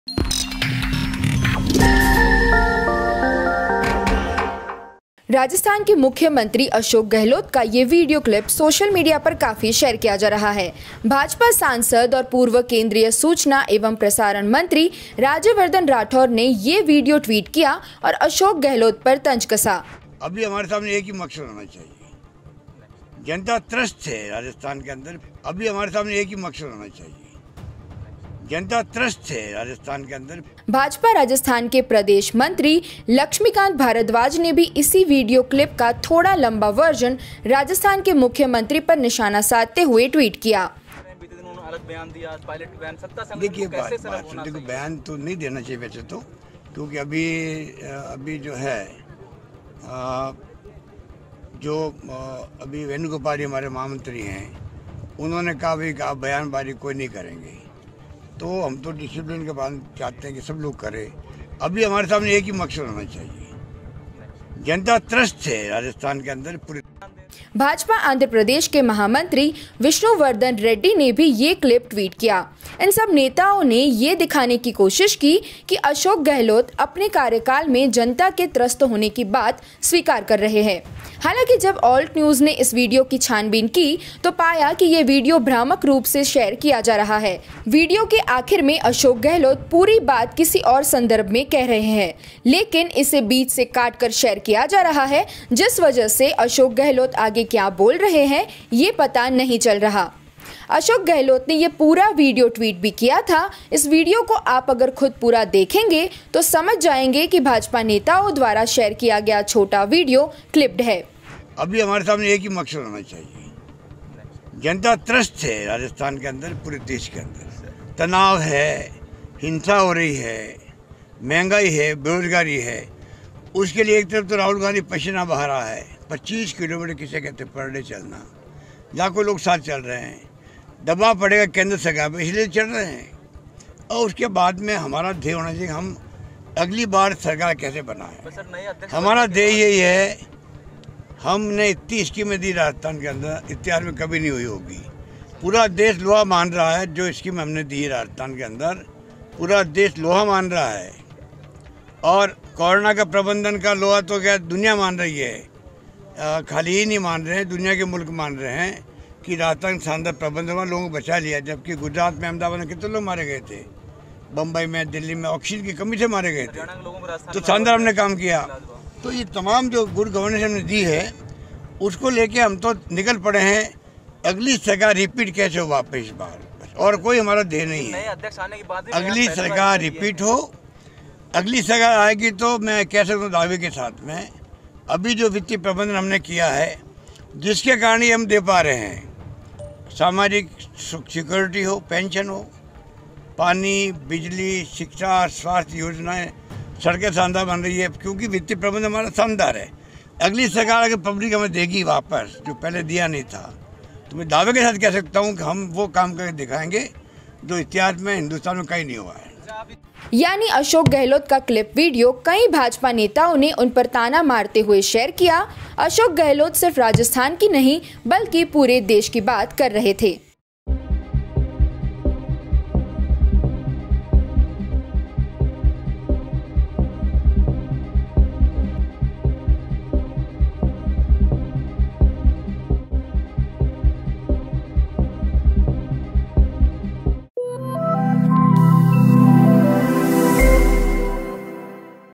क्लिप्पड वीडियो के सहारे अशोक गहलोत पर भाजपा नेताओं ने कसा तंज
A video of Rajasthan CM Ashok Gehlot is widely circulating on social media. In this clip, Gehlot can be heard saying that the people of Rajasthan are frustrated with Congress. In reality, the viral video is clipped.